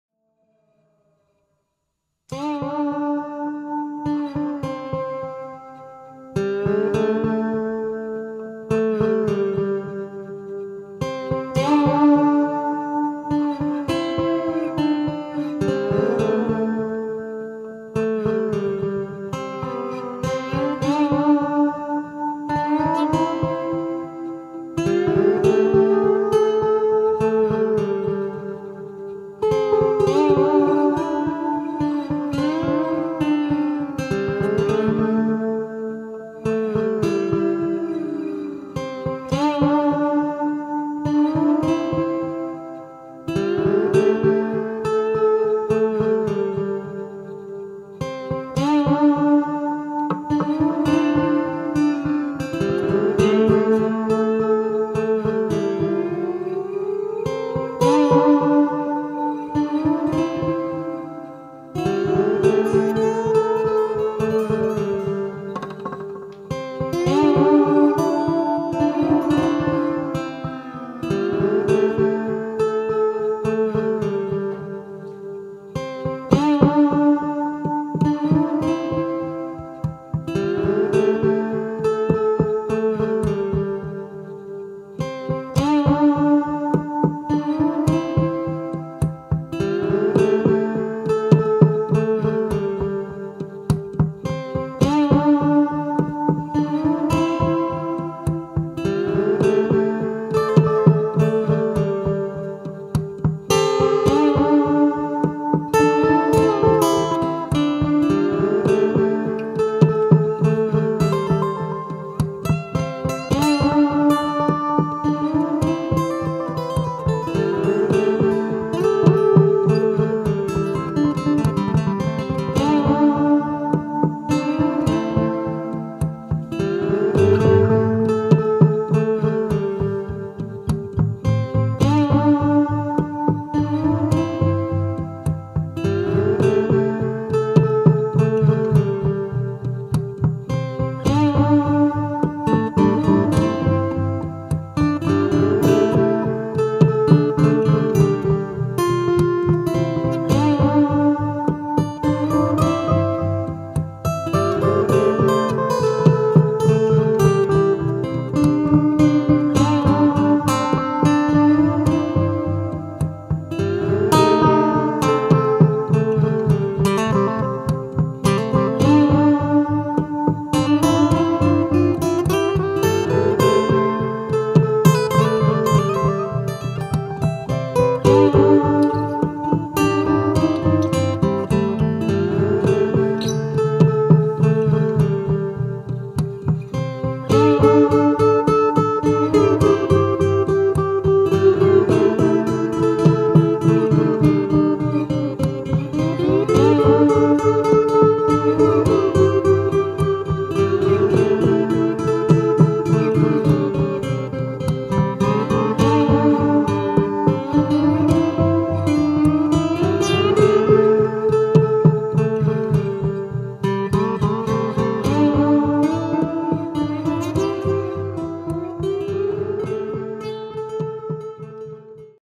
Восхитительный слайдер на гитаре (Импровизация)
Всё записано с первого дубля. Просто импровизация. Не претендую ни на что :)
Кстати, забегайте в нашу телегу и ВК, будем очень рады!